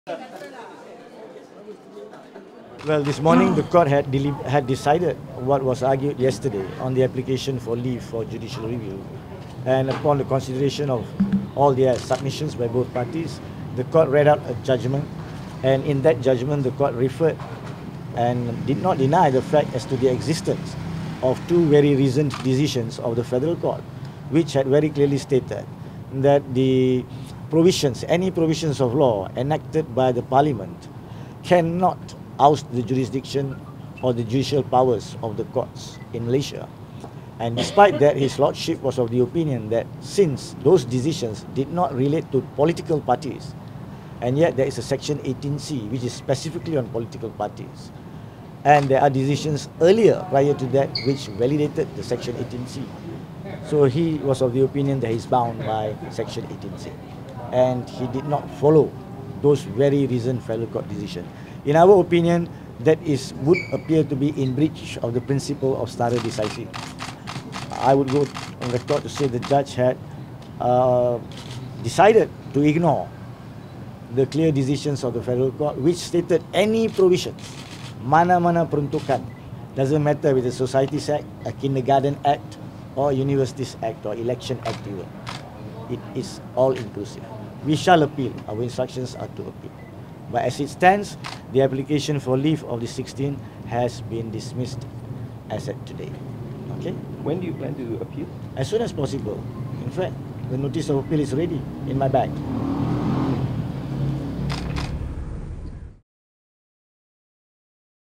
Court dismisses suit to dissolve Umno
The Kuala Lumpur High Court has dismissed the application for leave for a judicial review by16 former Umno members to dissolved the party due to the postponed internal election.

In delivering his decision, Justice Datuk Kamaludin Md Said said he had considered the questions raised by the plaintiffs, but felt that he was bound by Section 18C of the Societies Act.

Lawyer Mohamed Haniff Khatri Abdulla, who represented the 16, said he had received instructions to appeal the decision and said he would do so at the “soonest possible time”.